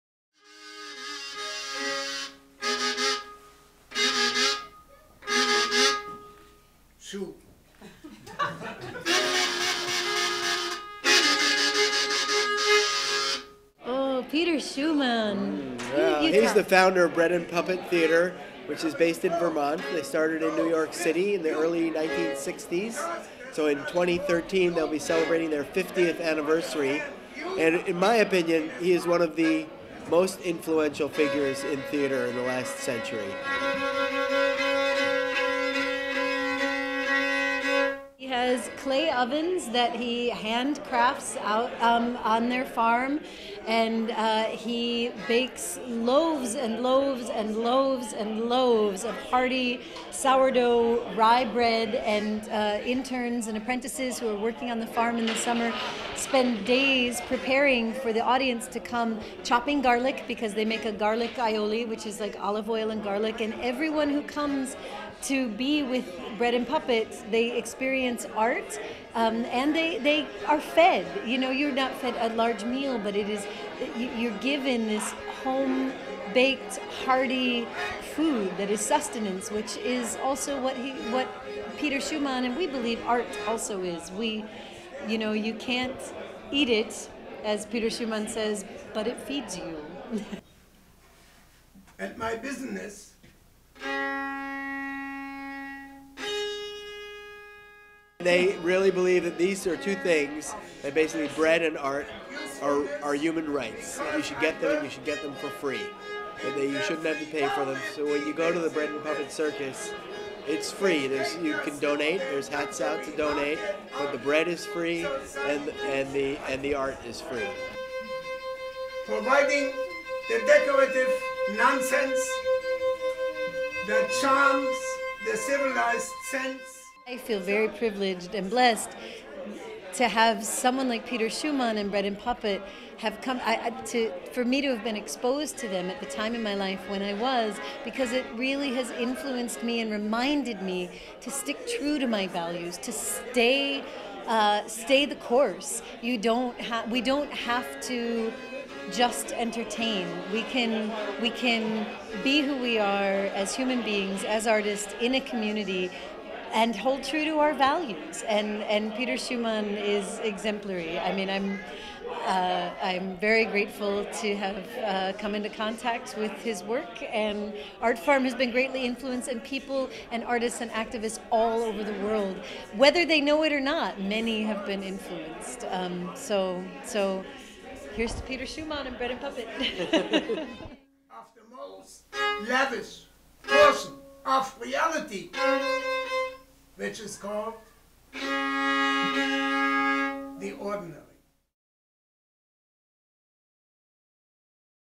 ArtFarm Welcomes Peter Schumann
The Founder of Bread and Puppet Theater, based in Vermont graced ArtFarm and Middletown, Connecticut with a special, once-in-a-lifetime performance. Dic and Marcella Wheeler show their appreciation.